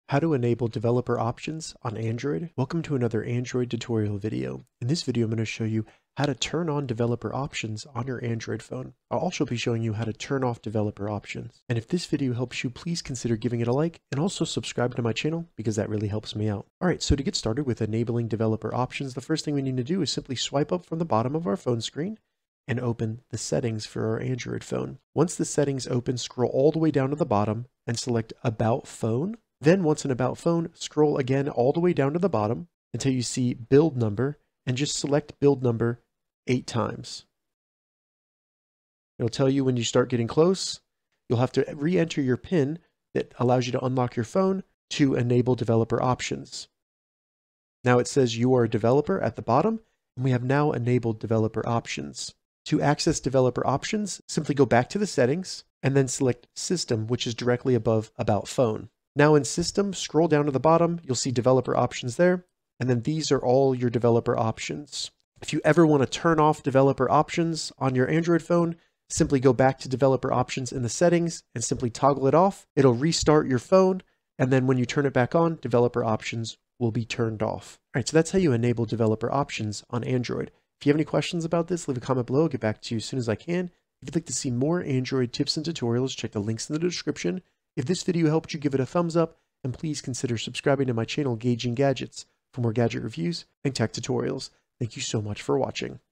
How to Enable Developer Options on Android - Turn On Android Developer Options
How to enable developer options on Android? In this full guide, I show you how to turn on developer options on an Android phone. This means you can open and change the developer options on your Android phone. This methods shows you how to enable developer options on the Pixel, Samsung Galaxy, OnePlus, Nothing Phone, Xiaomi, Realme, Moto, and any other Android phones.

Follow the steps below to Enable Developer Options on Android Phone:
1. Open the settings on your Android phone or tablet.
2. In the settings scroll down and select About phone.
3. Scroll down and select Build Number seven times in a row.
4. Enter the passcode for your Samsung phone.
5. Go back to the main page of the settings and then scroll all the way down and select Developer Options.